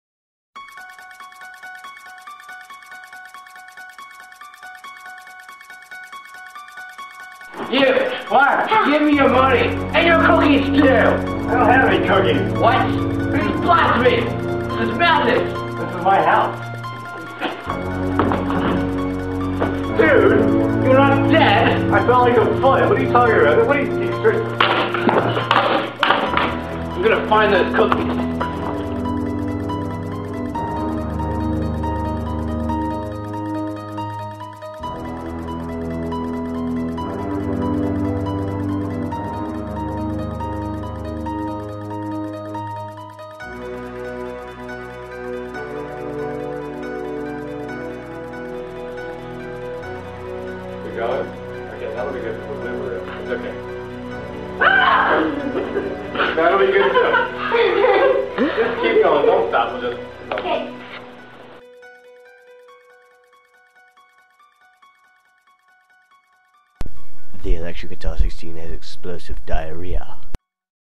A Day in the Life of a Thief - Cookies
When a thief decides to rob a man, he doesn't just want money . . . you'll see.